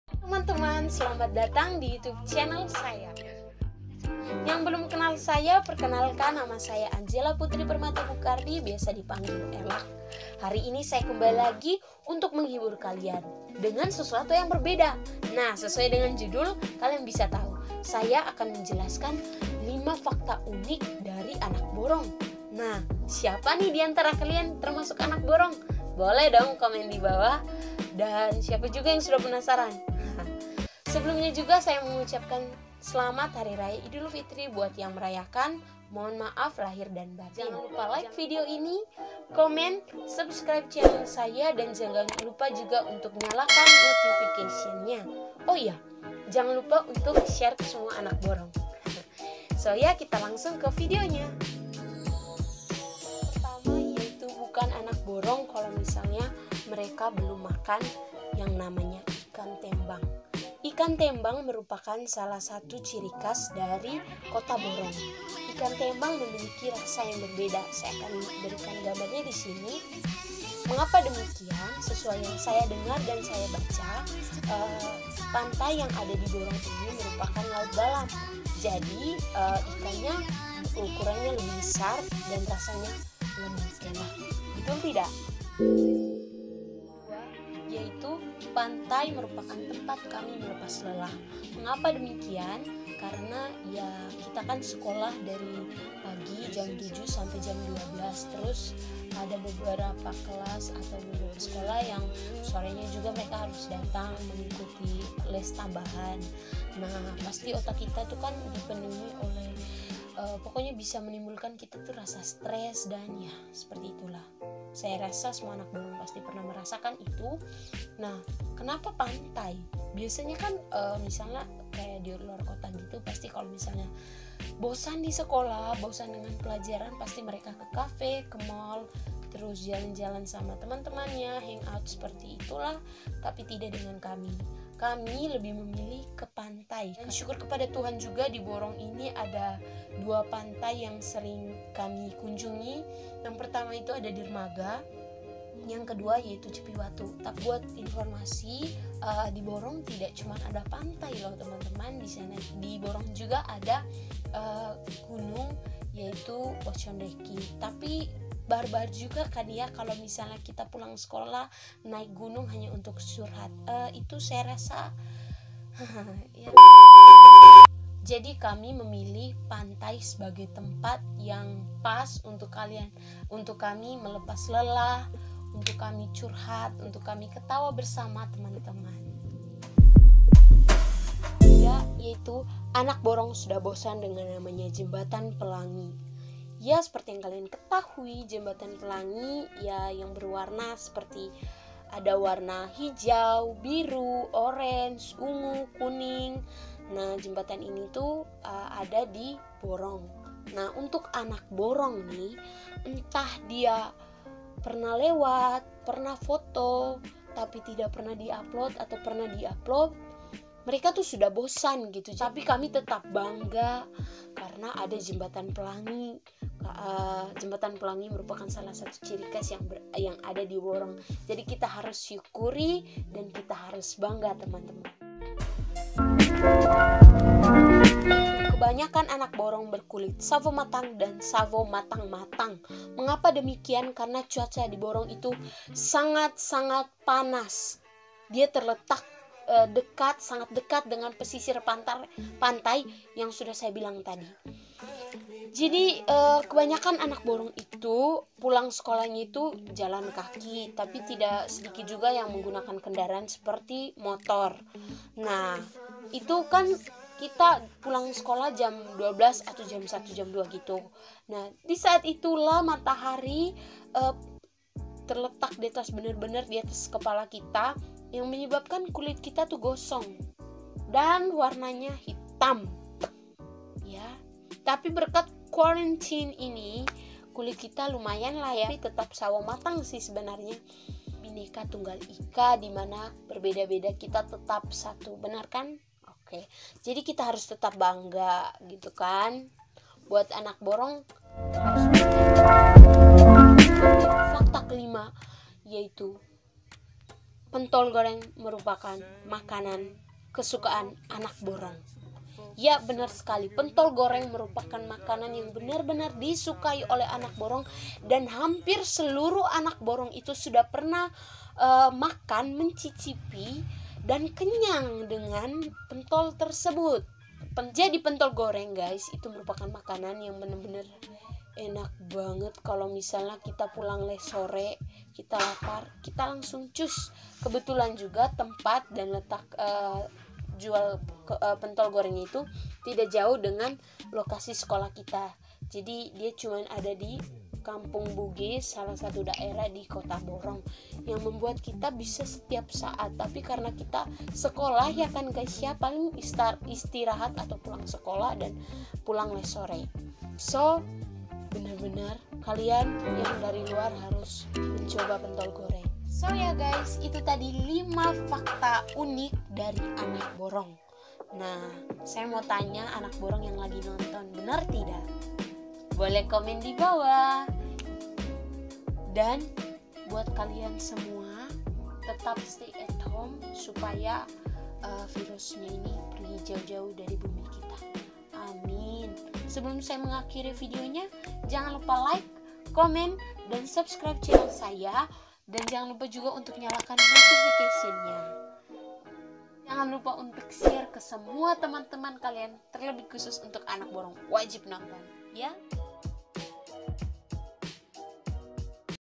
5 Fakta Unik dari anak Borong!? | yang ke-5 Bener-bener fakta!😂
hallo!✨
5 fakta unik dari anak borong! Siapa ni yg termasuk anak Borong😁 semoga kalian setuju yh😂

ILY❤✨